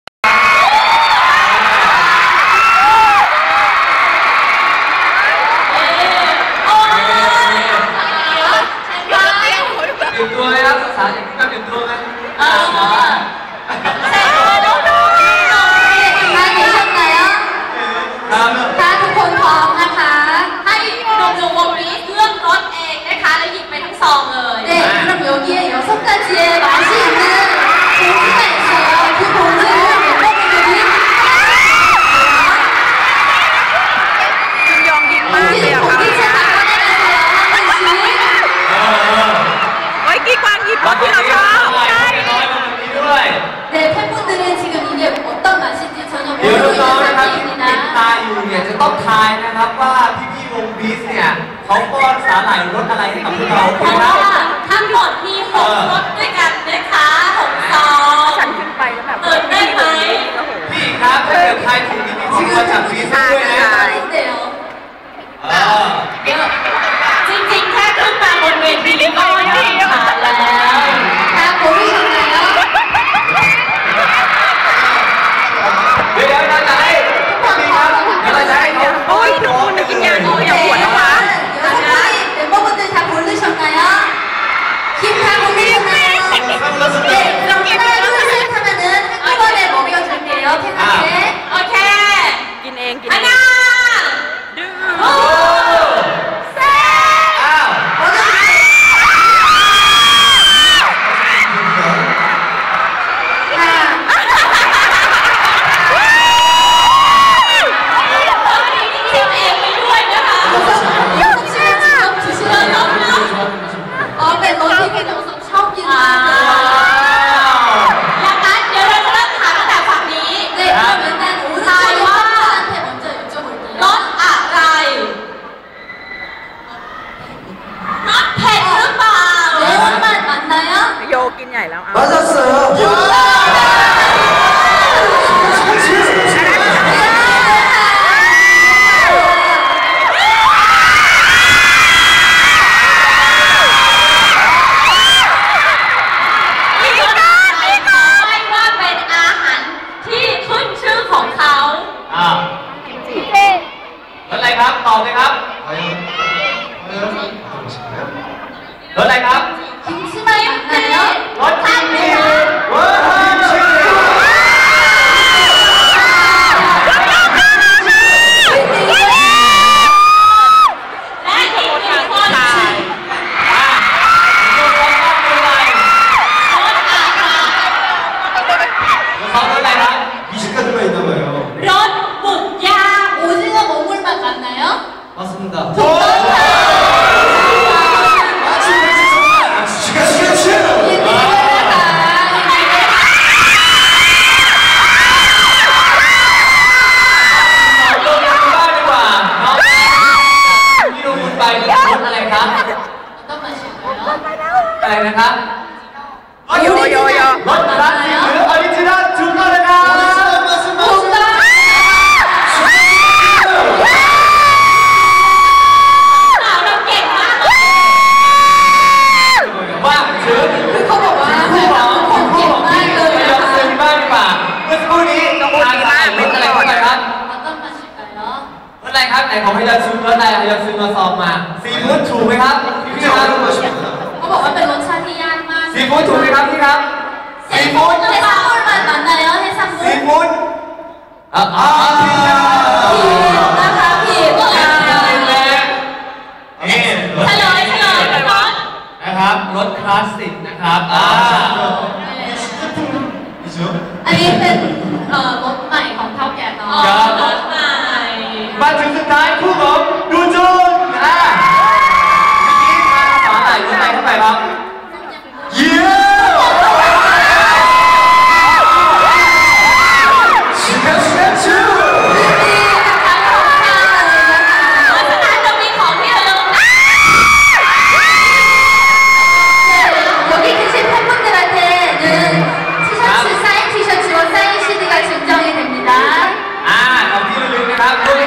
[Fancam] Beast - Playing Game @ EXCLUSIVE FANMEETING 'B2ST AND B2UTY' IN THAILAND [120114]
(Rec. from my cellphone It's not clear)